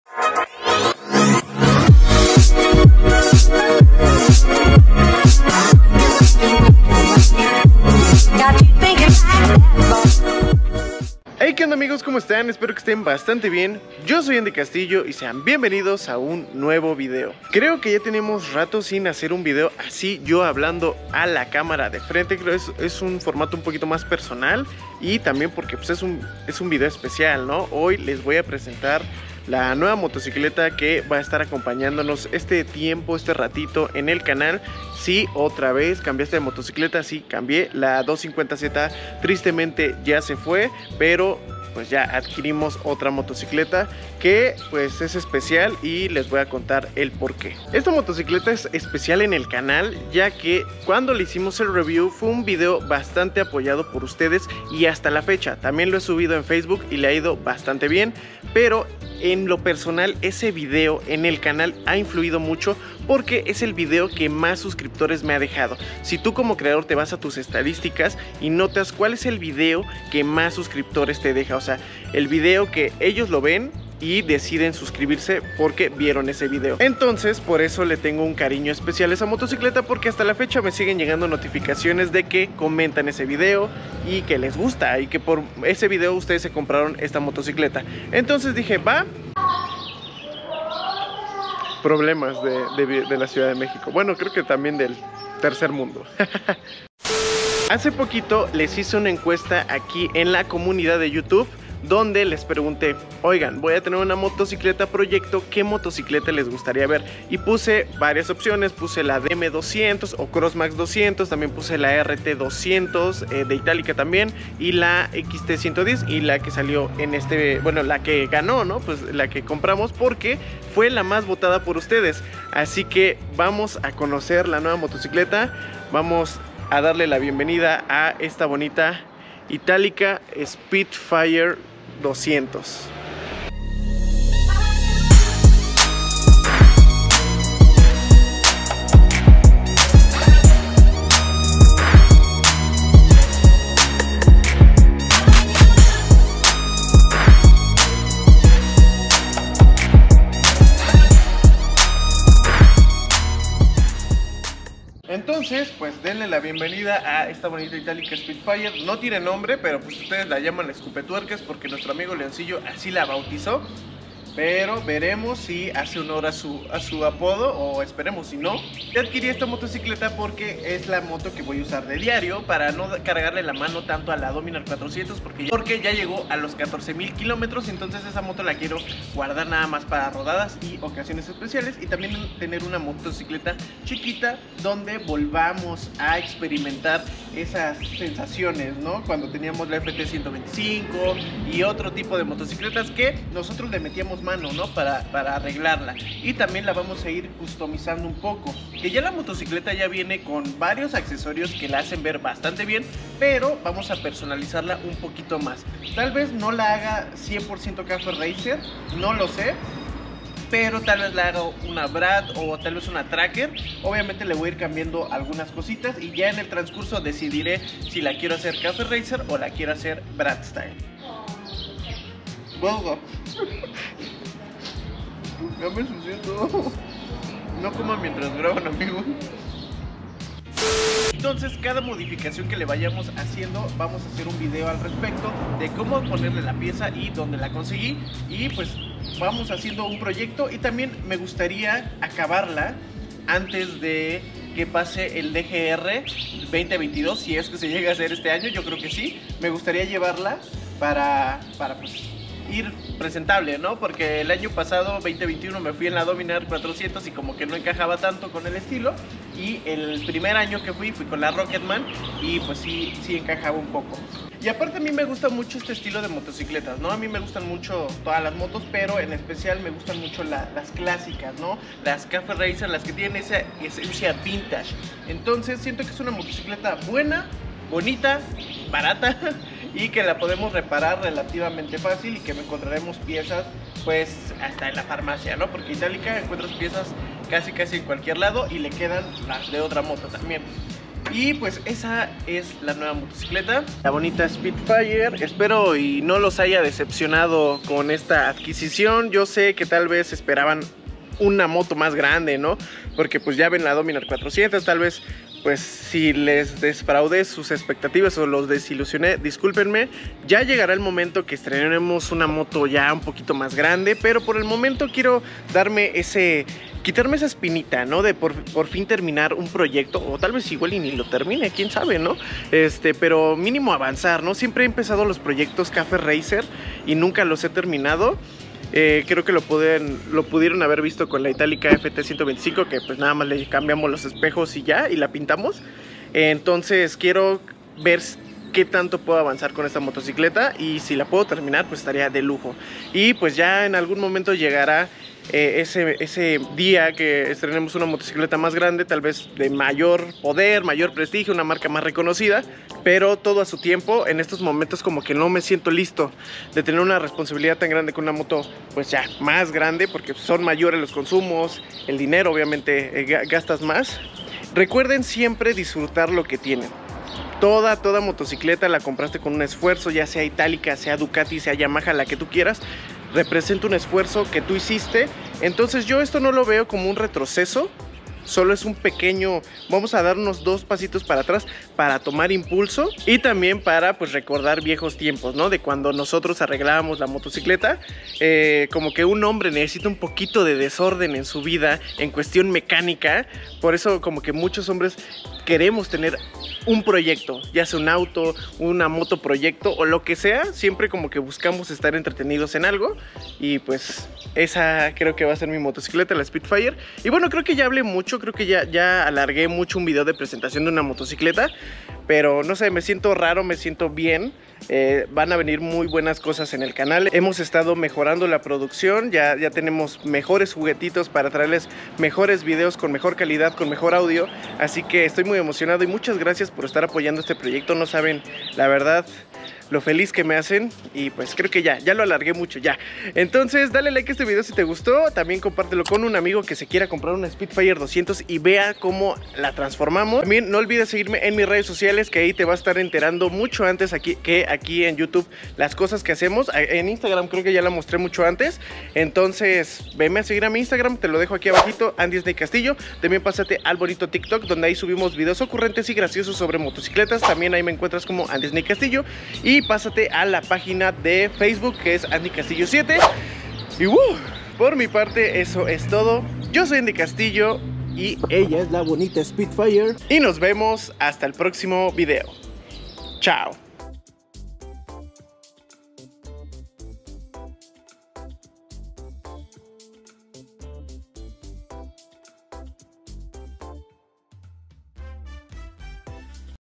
🔥Esta es mi NUEVA MOTO🔥 (Si otra vez ) 😅
Si otra vez, no es un deyabu 😂
volvimos a cambiar de motocicleta
y en este video la conocerán